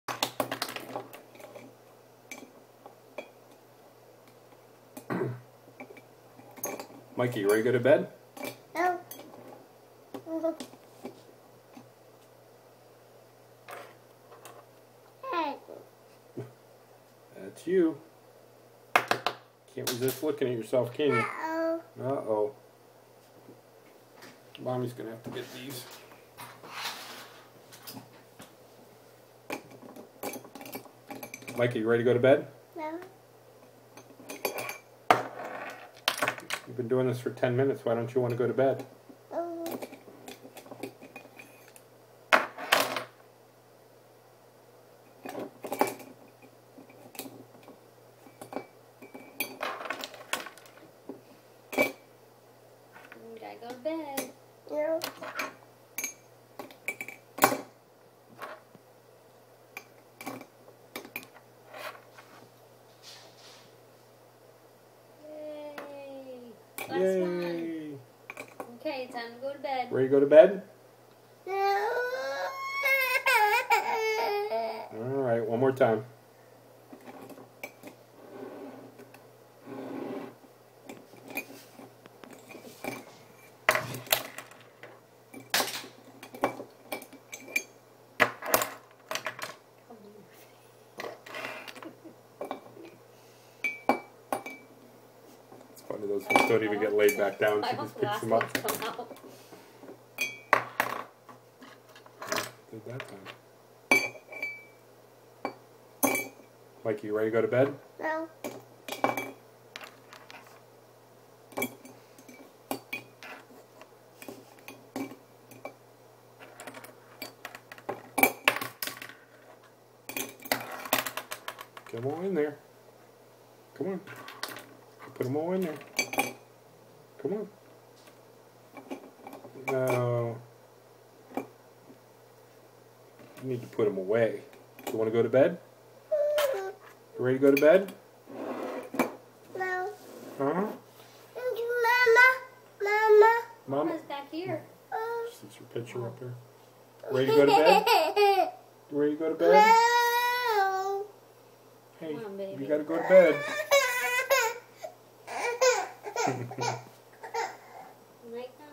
End of night routines
Ever night before bed she sits in my lap at the computer and plays with "stuff" on my desk. Her new thing is taking all the pens out of the cup and putting them back in...at least 20-25 times!